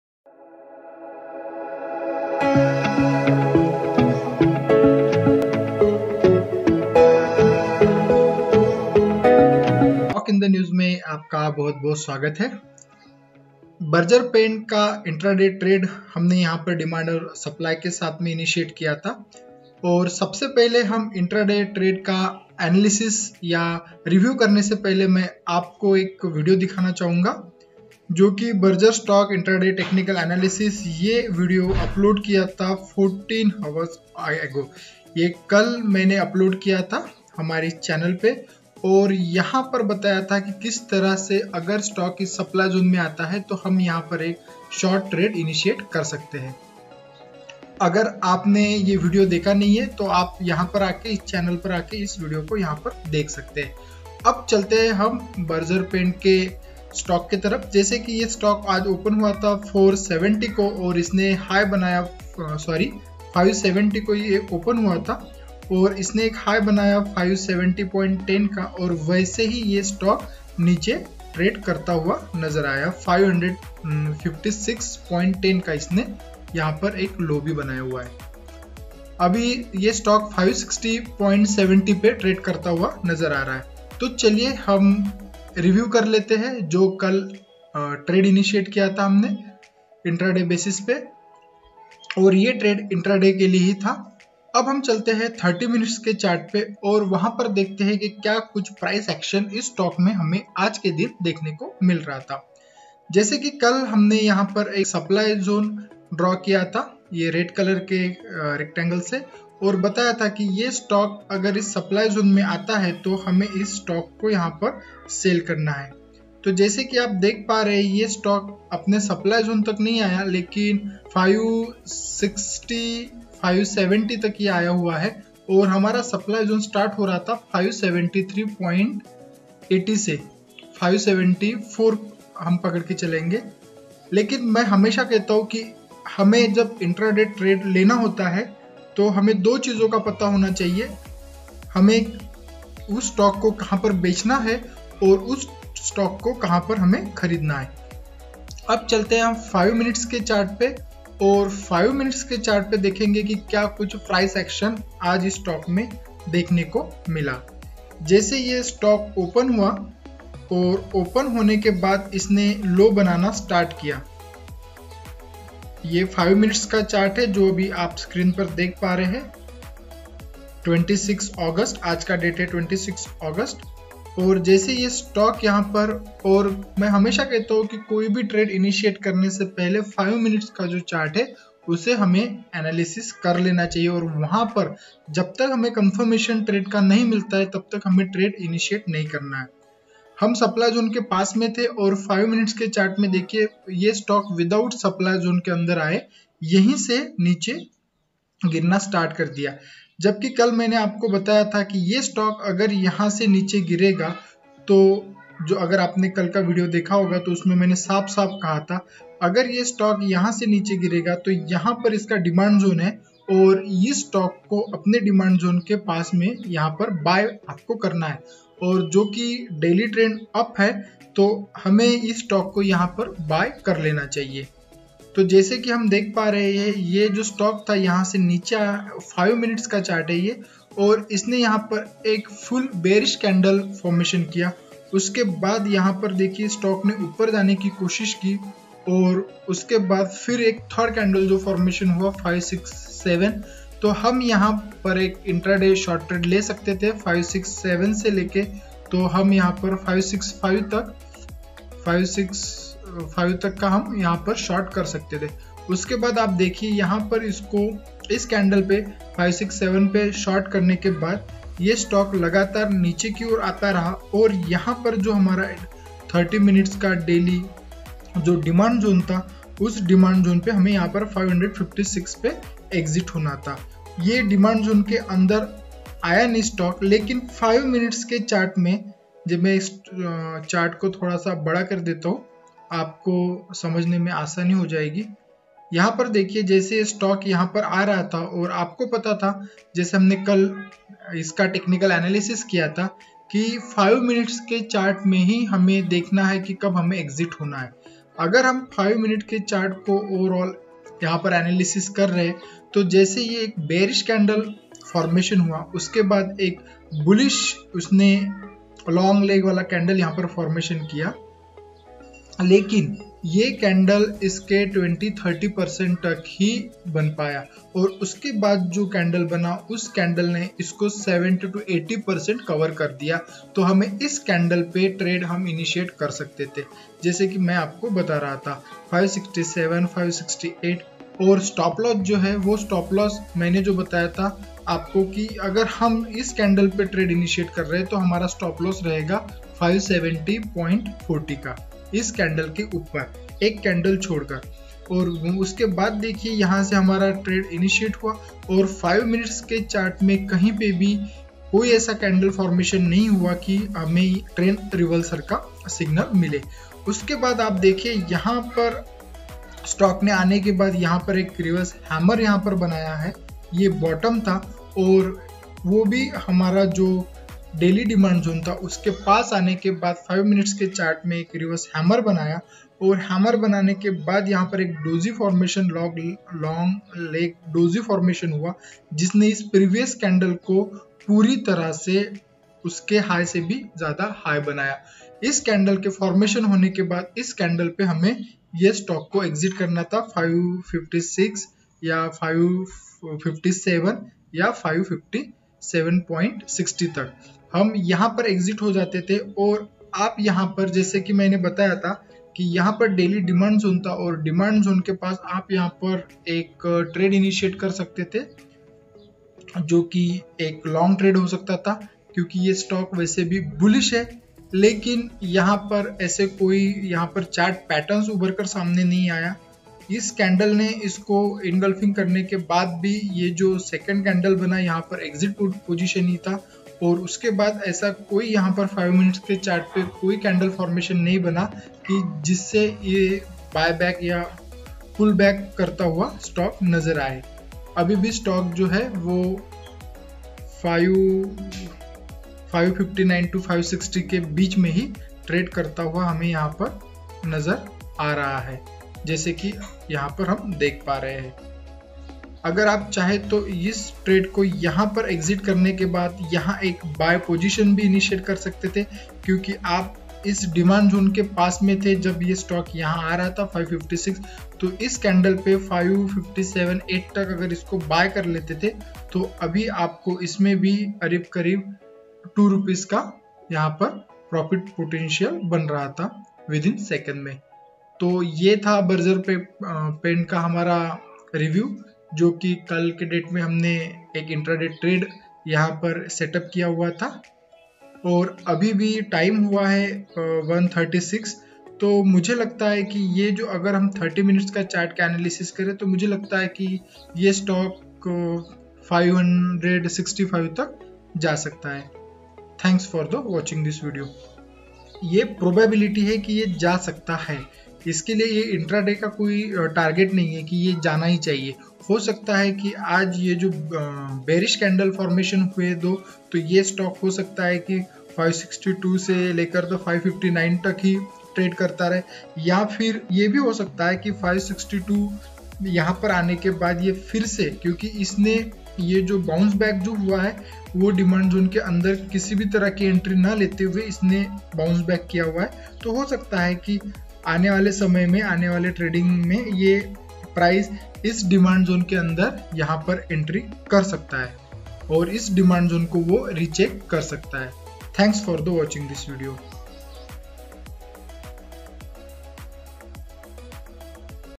BERGEPAINT Stock Intraday Demand and Supply Trade Review
This video is to learn you how to find out demand and supply zone. BERGEPAINT Stock Intraday Demand and Supply Trade Review. Demand and supply is a very big concept and this concept is also working in the stock market.